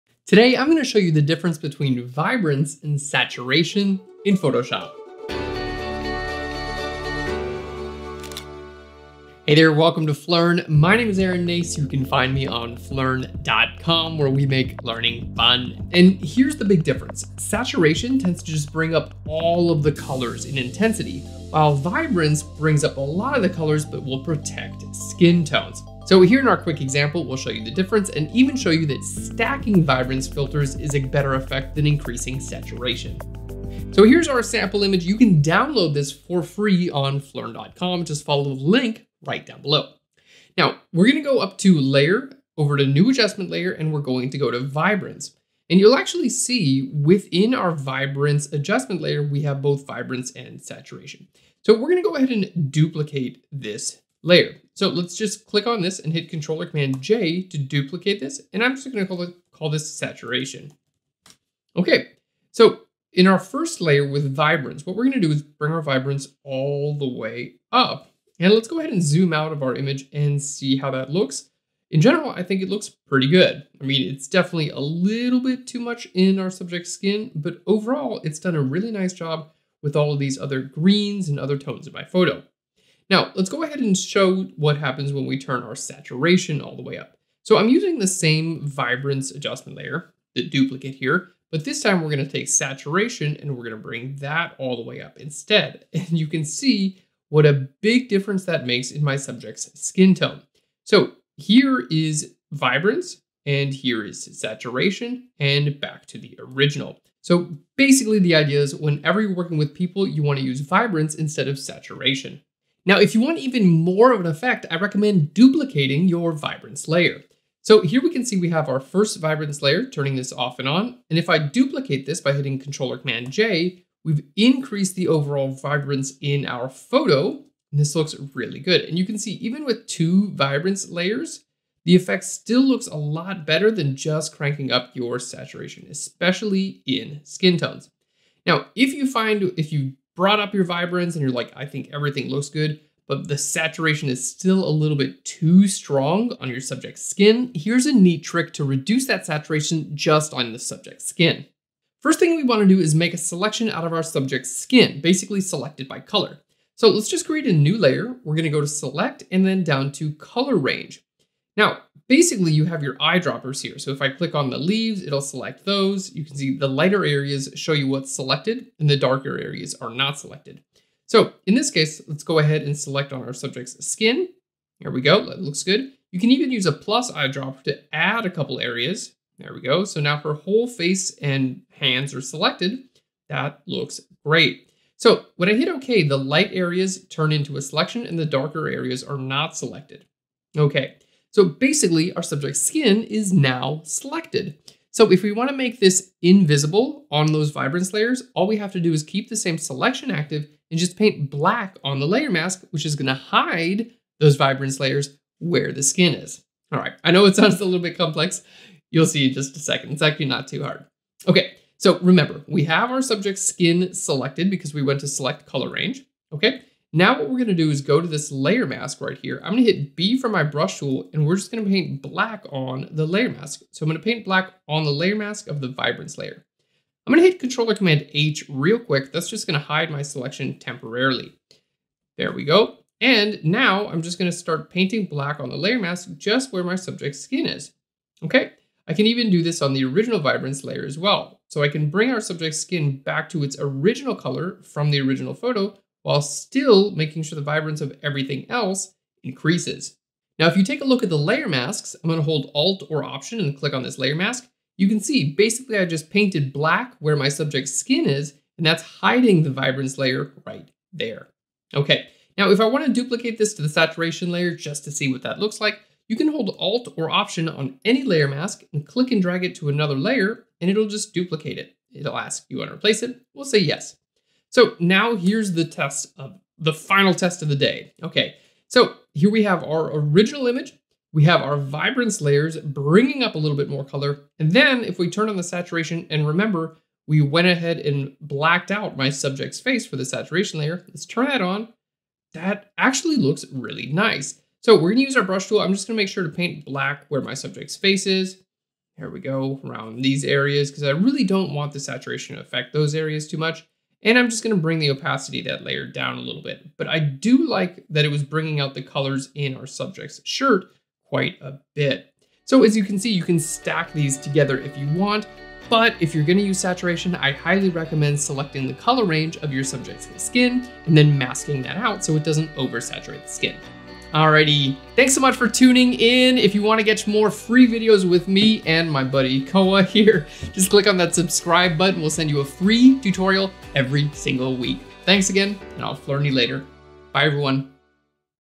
Saturation vs. Vibrance: What's the Difference? | Photoshop Tutorial
Today we break down the difference between Vibrance and Saturation in Photoshop! Learn how to enhance the colors in a photo without affecting skin tones, stack multiple adjustments to dial-in the perfect look, and use Select Color Range and Layer Masks to add even more precision to your work.